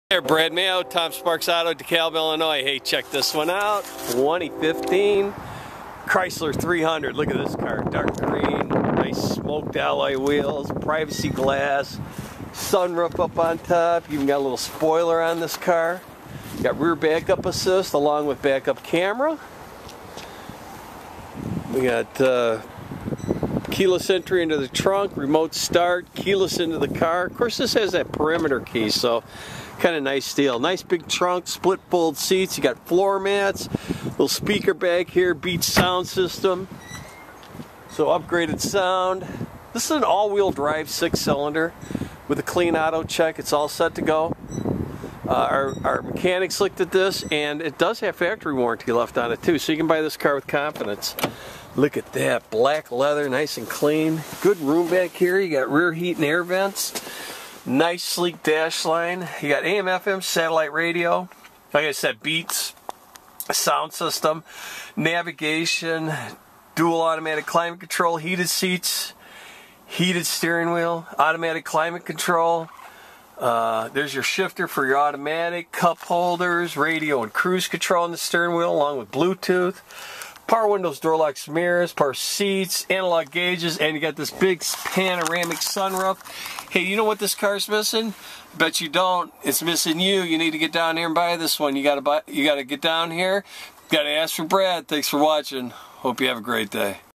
2015 Chrysler 300S AWD Dekalb IL near Malta IL
ONE OWNER CLEAN AUTO CHECK EQUIPPED WITH; NAVIGATION SYSTEM, POWER SUNROOF, LEATHER SEATS, HEATED SEATS, AWD, REMOTE START, PREMIUM SOUND SYSTEM AND MORE. CALL BRAD FOR AVAILABILITY.